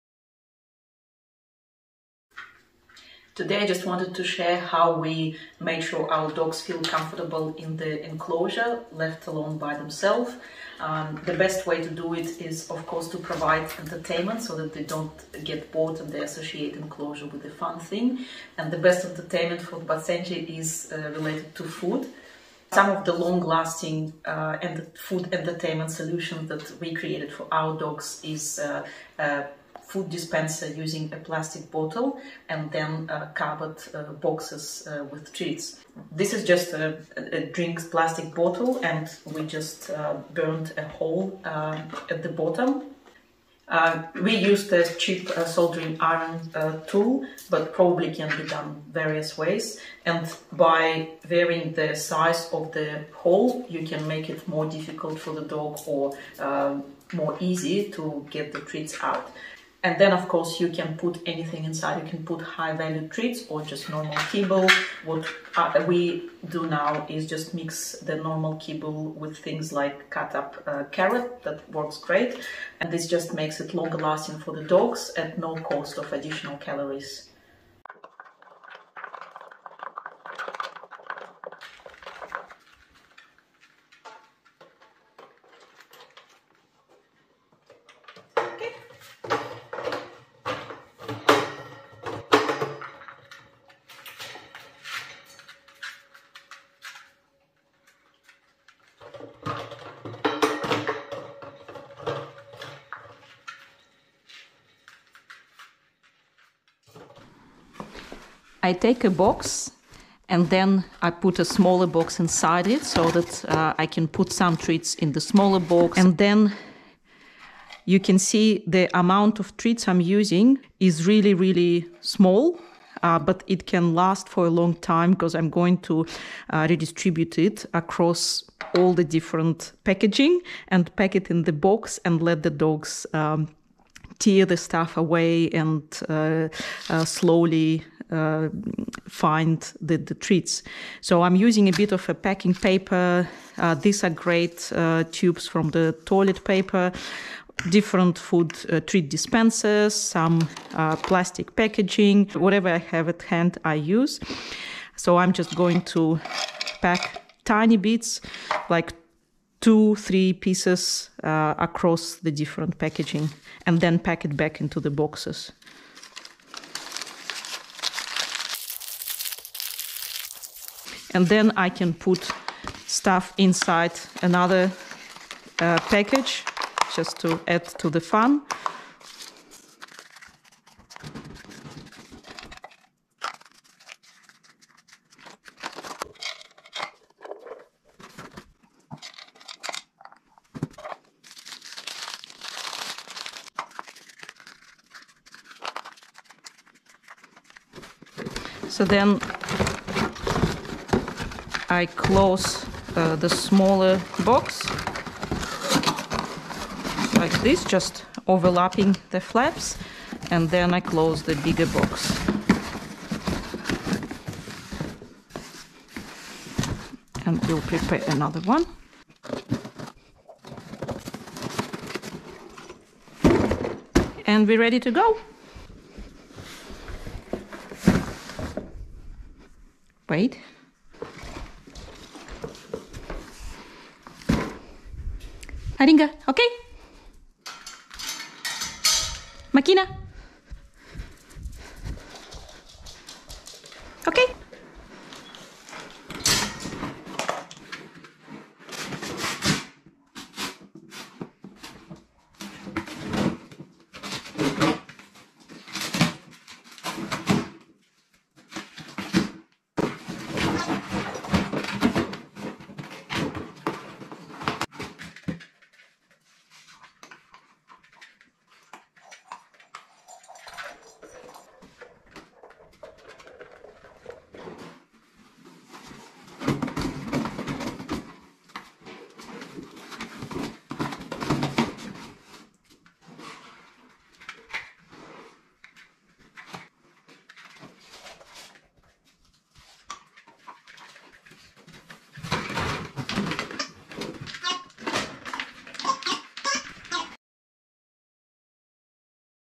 How to occupy basenji locked in an enclosure - long lasting food entertainment
Two enrichment solutions for basenjis to make them content with being locked in an enclosure or playpen:
00:00 Intro
00:36 Plastic bottle food dispenser
01:47 Cardboard boxes with hidden food